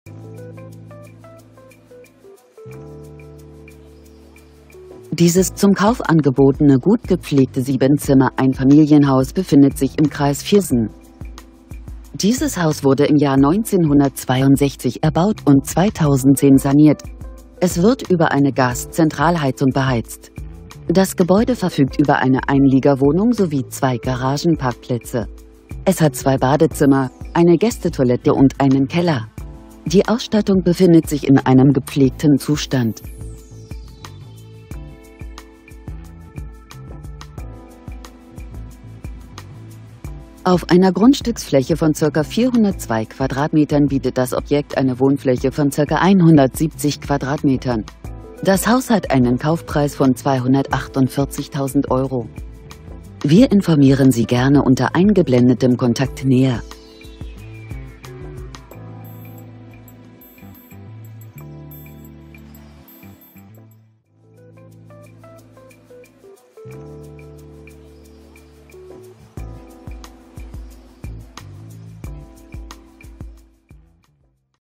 Großzügiges Wohnhaus in guter Lage von Grefrath zu verkaufen.
Objektnr.: 2016
Informationen zum Energieausweis:
Baujahr: 1962
Heizungstyp: Zentralheizung
wesentliche Energieträger: Gas